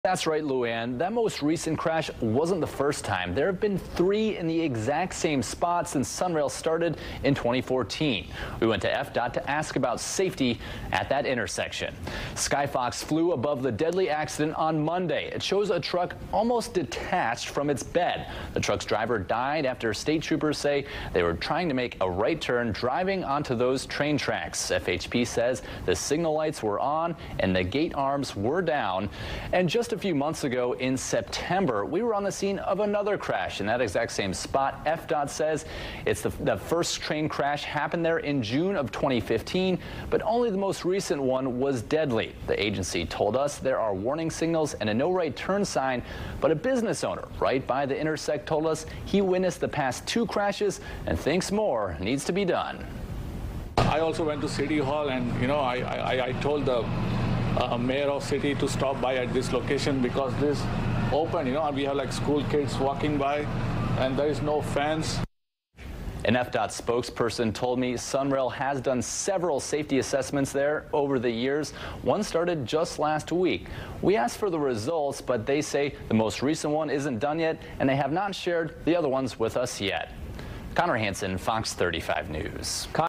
Sanford SunRail station prompts safety concerns after deadly crash
Transportation officials have given FOX 35 more details about an intersection where trains have crashed into cars three different times.